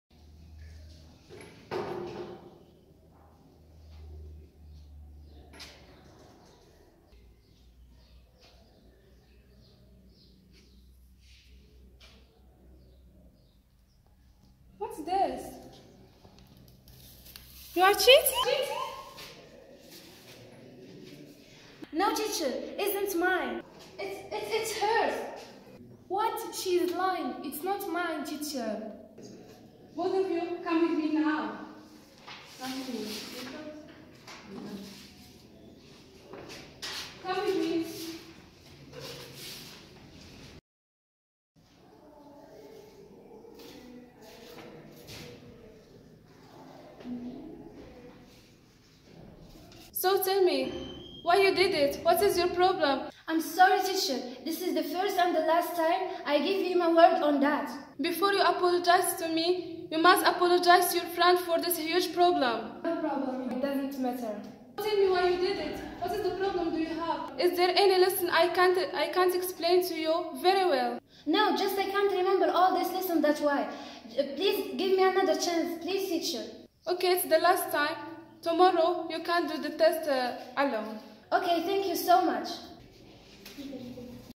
Goulmima Video Project: Testing the Teacher
Group Name: The Great Leaders
This is a student video project. Students were given the freedom to create their own story and script. They all chose topics that were of interest and important to them.